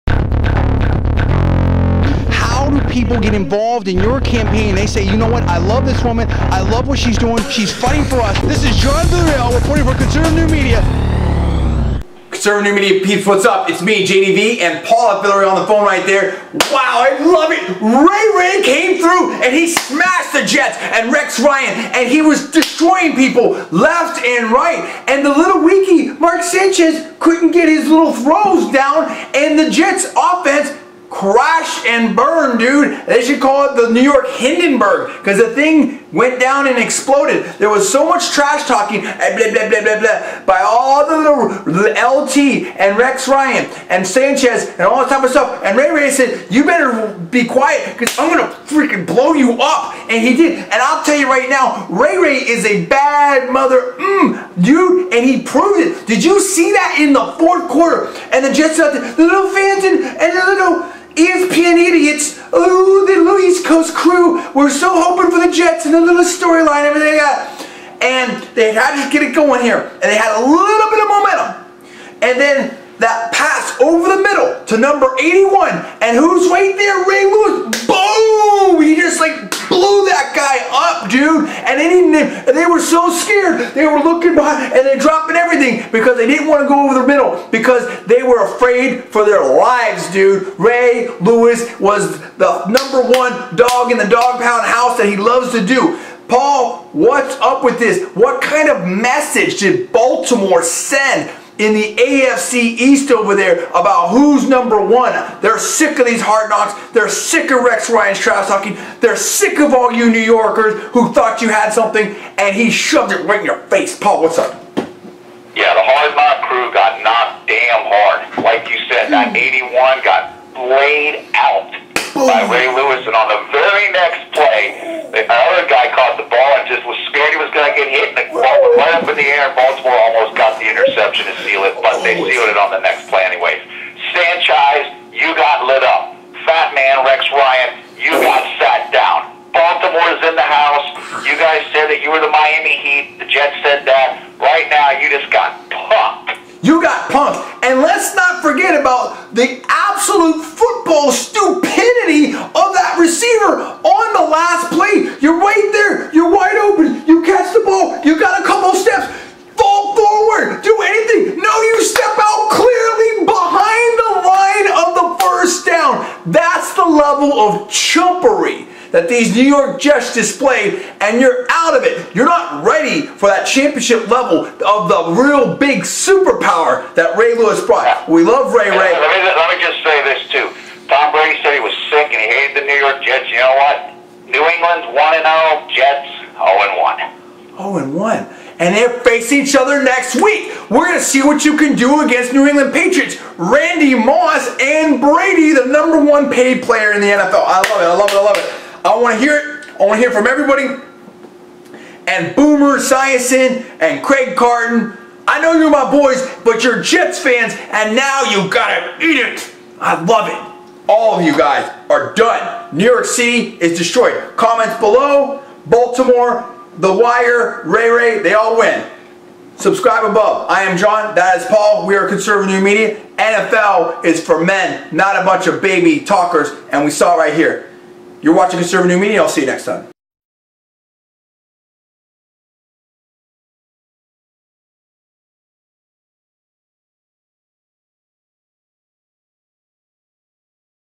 Ray Lewis & Baltimore Ravens DESTROY NY Jets on Monday Night Football!
Ray Lewis & the Baltimore Ravens DESTROY the NY Jets on Monday Night Football!

CNM breaks down the Ravens' 10-9 win and gives you the details on who got punked and who came through in the game!